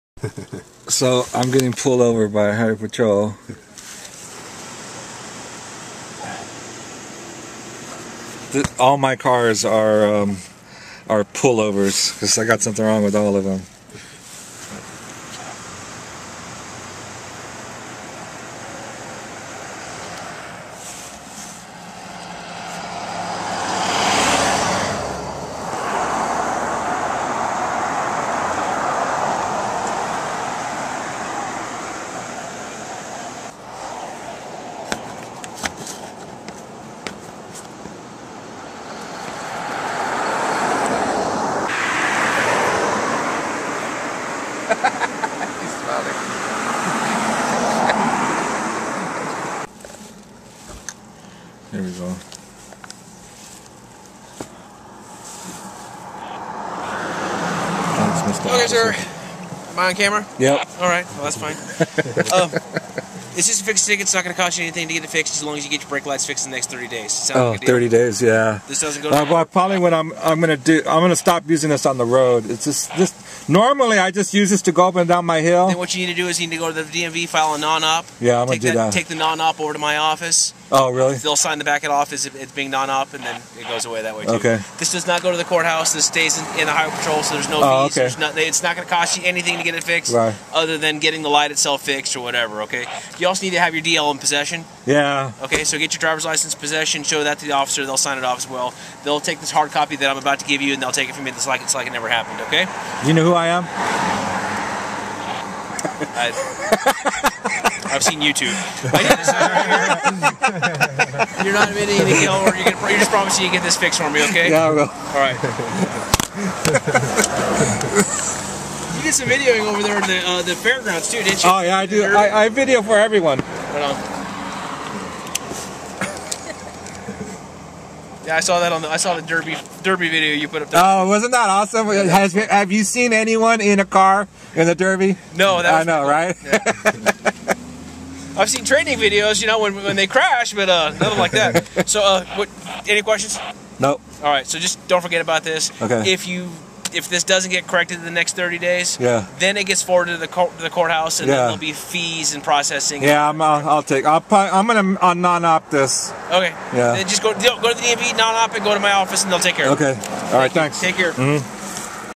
Smokey Bear
I got pulled over by CHP and got a ticket for no tail lights and no drivers license in possession.